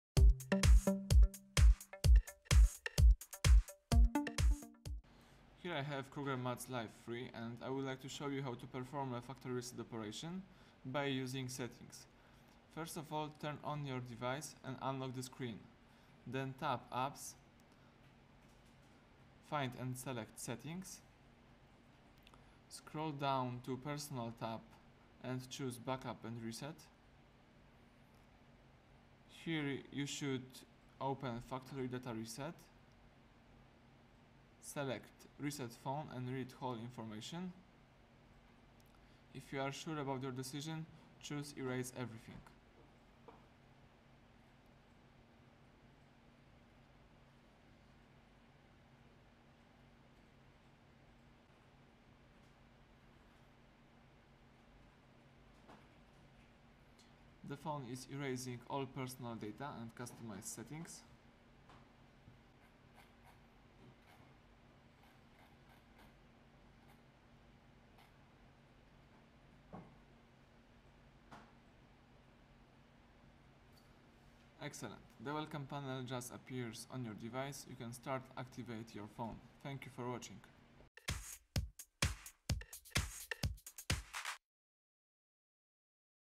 Hard Reset KRUGER & MATZ LIVE 3 - wipe your Android device
How to accomplish the factory reset? How to erase everything from your device? How to reste your phone? How to perform a factory reset?
A factory data reset can fix most issues that can occur on an Android devices. This process will clear the entire phone and revert it back to how it was when it came off the assembly line.